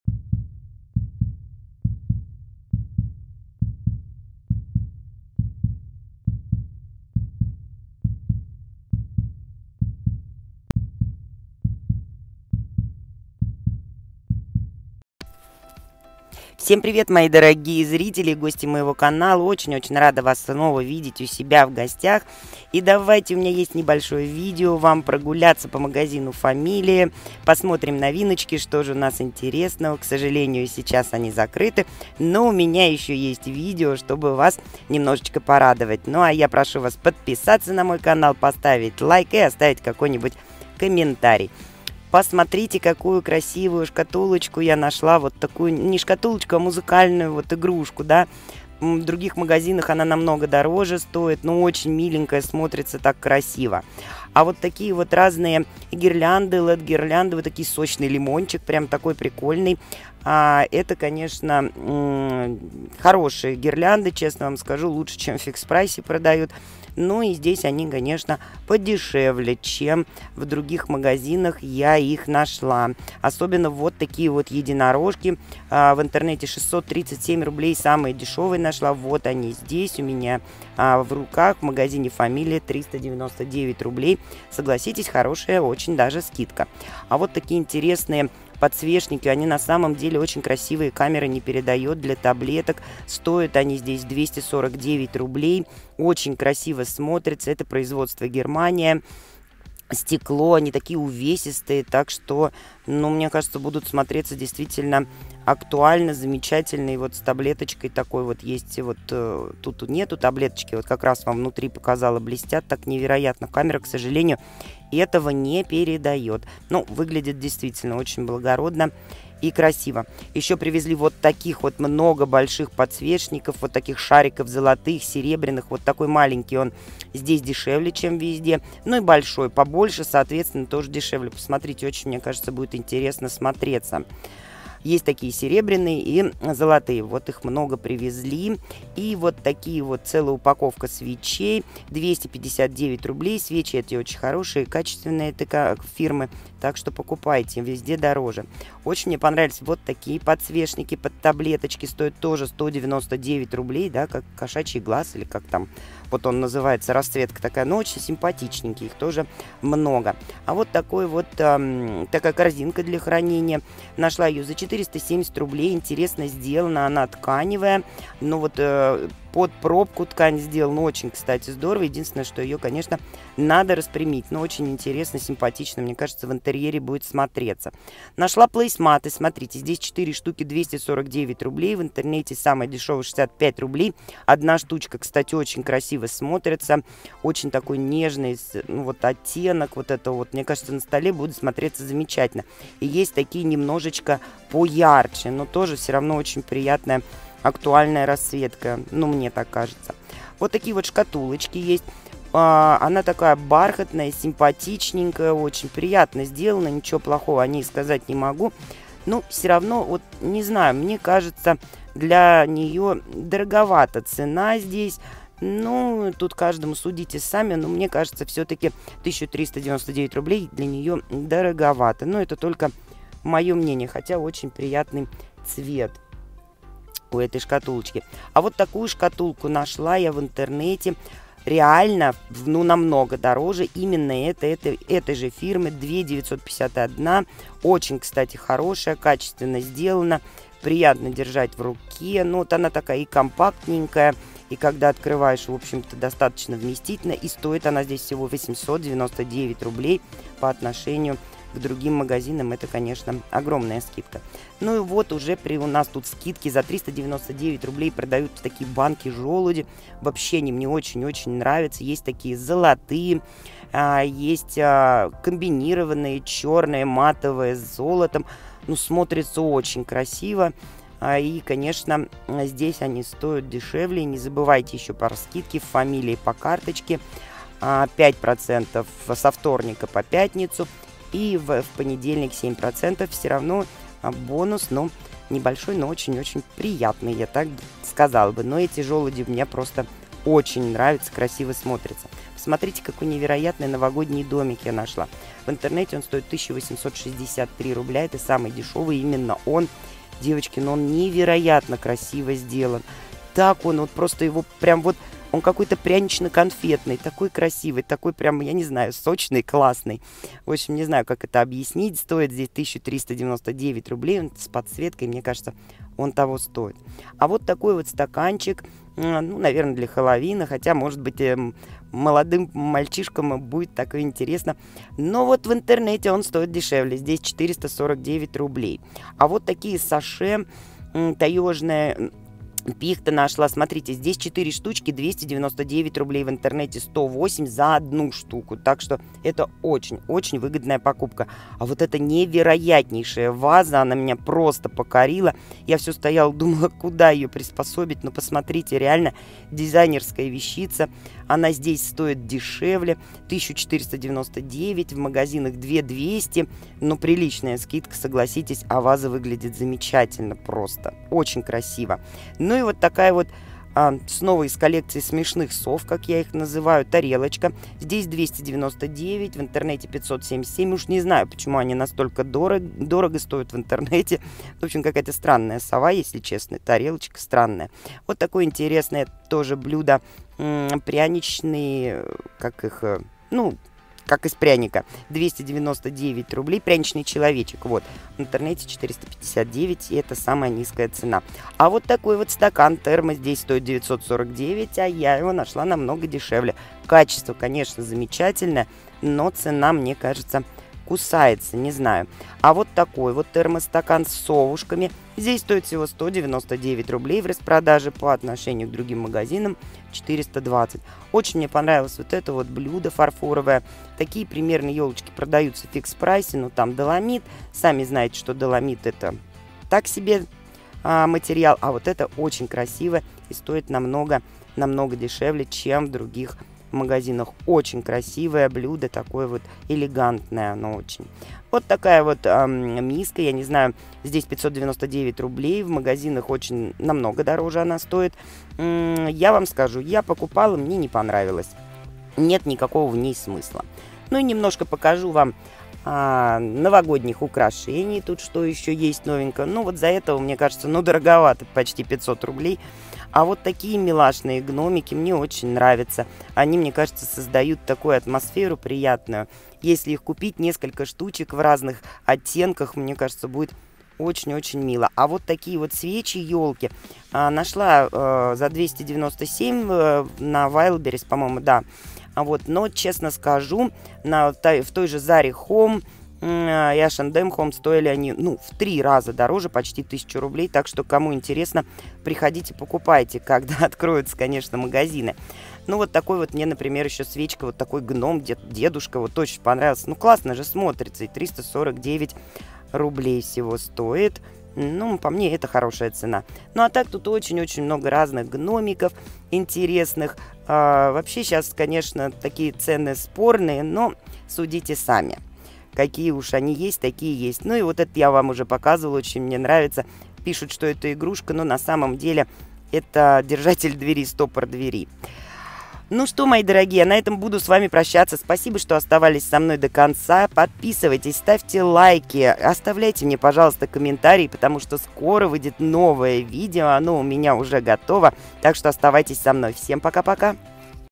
ФАМИЛИЯ МАГАЗИН! ШОК НОВИНКИ! НЕ ПРОПУСТИТЕ! Цены 🔥 огонь!
Всем привет! выкладываю для Вас, мои дорогие,  видео обзор Магазина Фамилия! И давайте посмотрим какие новинки на этой неделе нам привезли! Всем спасибо что смотрите меня! Что остаётесь со мной и подписываетесь. Этим Вы поддерживаете и развиваете мой канал! Ещё раз спасибо!

Магазин находится по адресу: Таллинская 18. Москва!